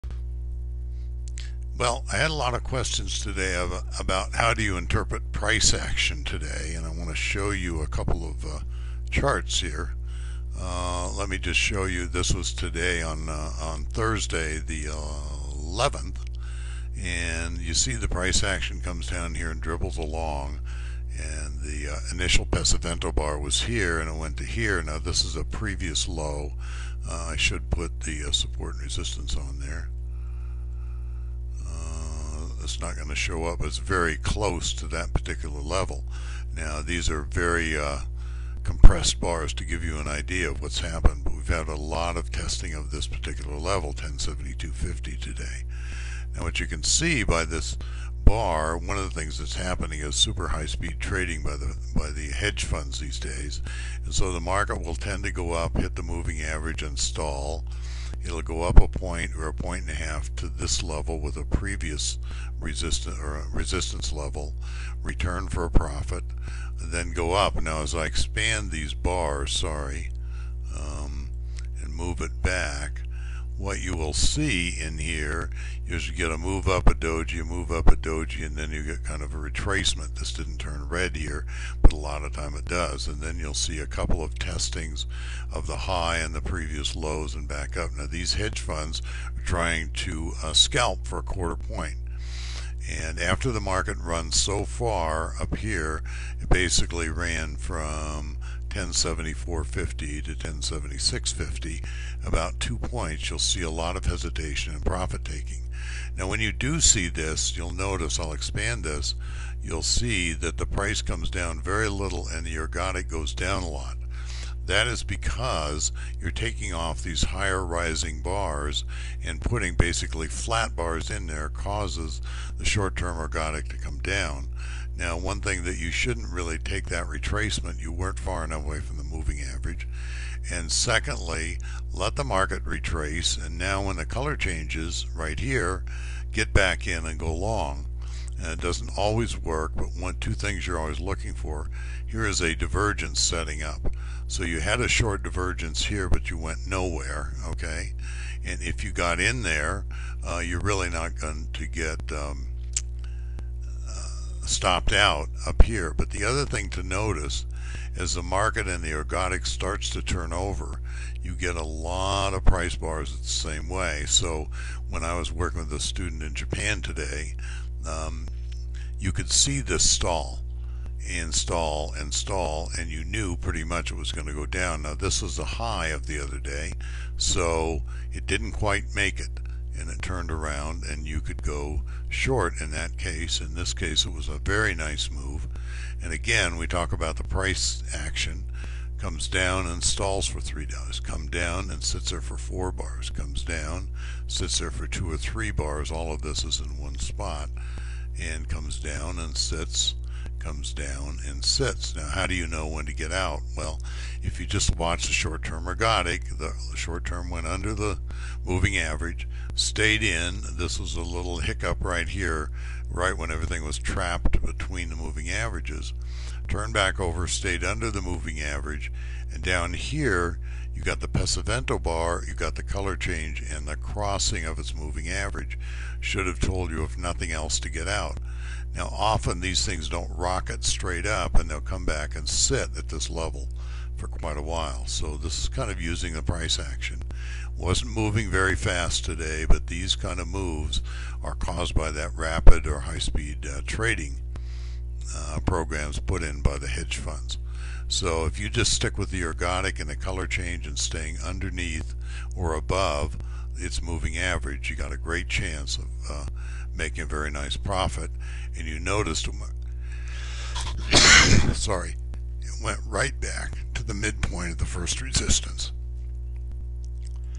How High Frequency Trading Affects Price Action
Advanced techniques for trading futures, forex, commodities and options.  How High Frequency Trading Affects Price Action